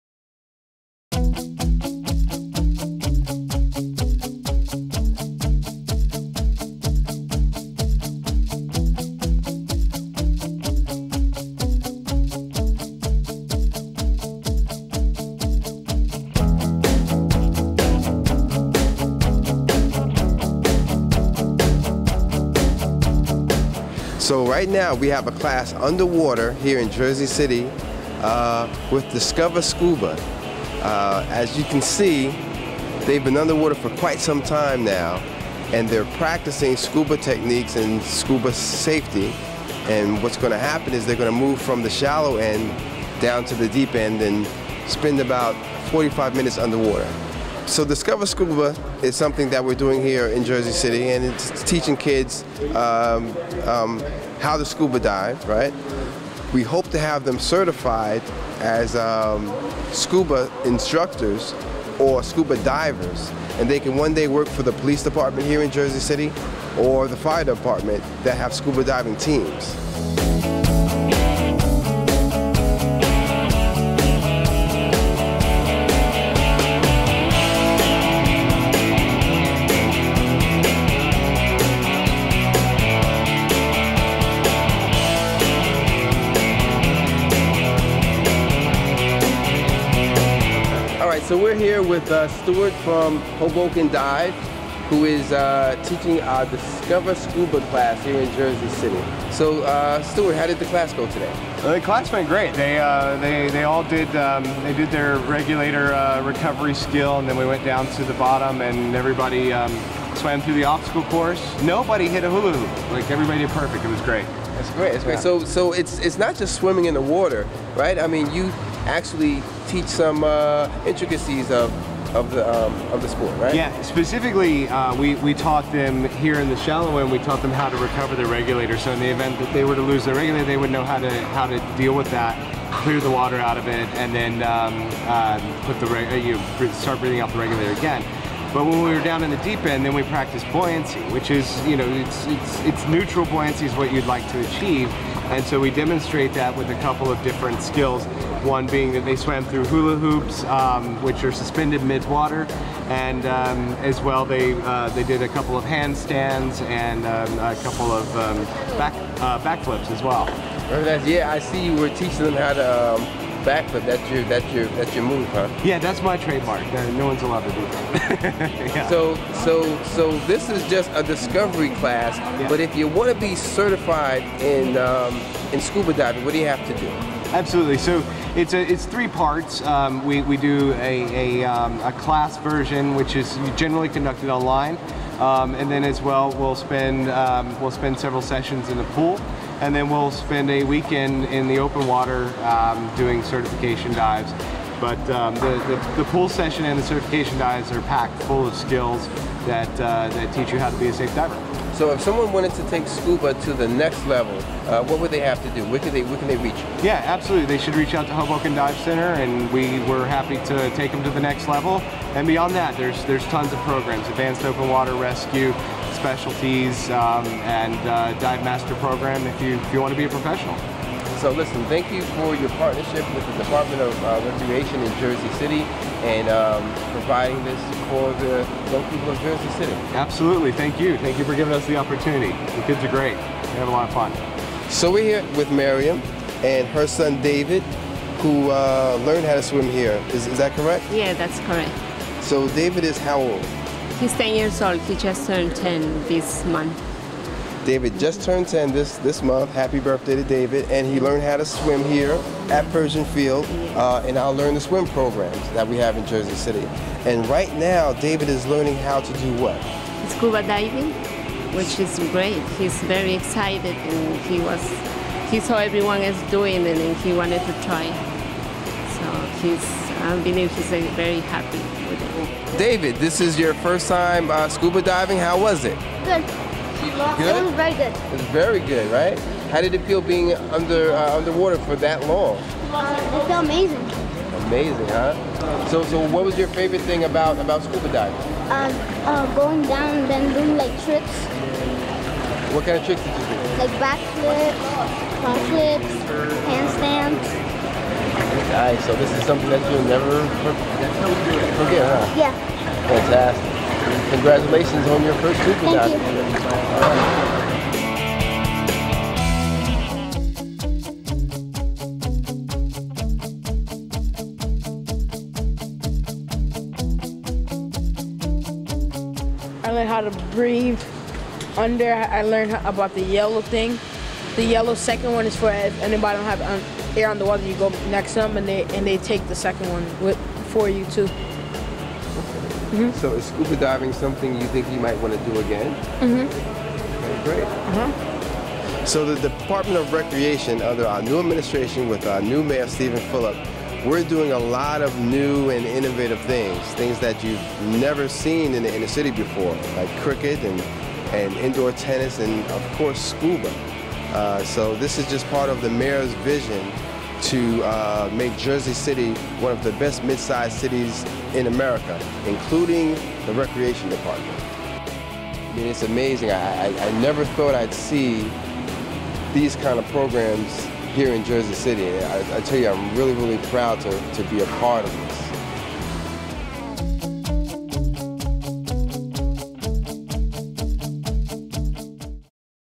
Recreation Discover Scuba Program
The Jersey City Recreation Department has started a Discover Scuba program to all Residents.